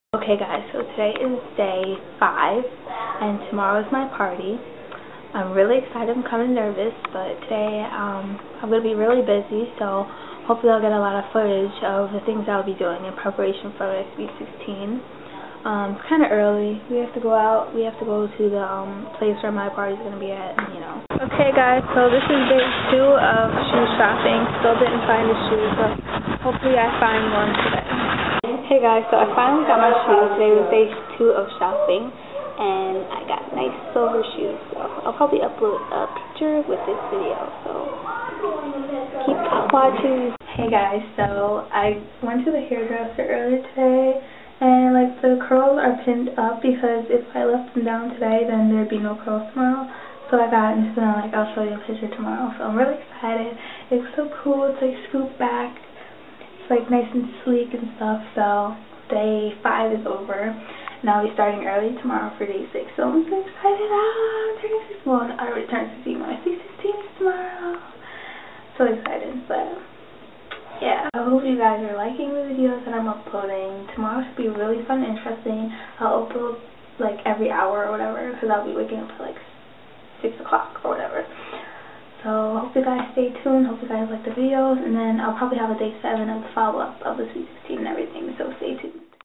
Day 5- My sweet 16 story
Ahhh today is day 5 tomorrow is my sweet 16!!
I have to wake up so early!
Today i got my hair done and its gonna look soooo cute :)
xoxo